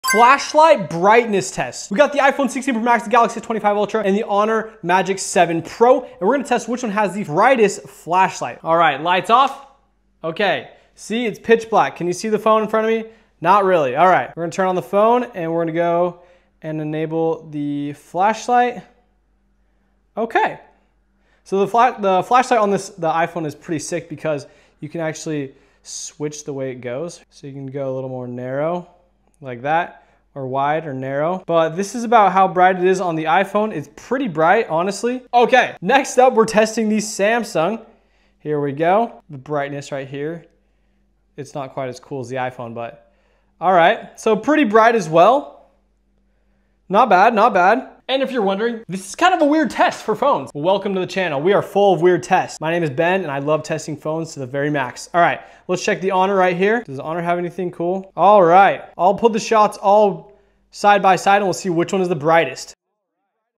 Galaxy S25 Ultra vs Honor Magic7 Pro Macro Flashlight Brightness Test!
In this Galaxy S25 Ultra vs Honor Magic7 Pro macro flashlight brightness test, we compare how each flagship performs in extreme close-up shots using their built-in flashlights. See which smartphone delivers the brightest, clearest, and most detailed macro shots in low-light conditions!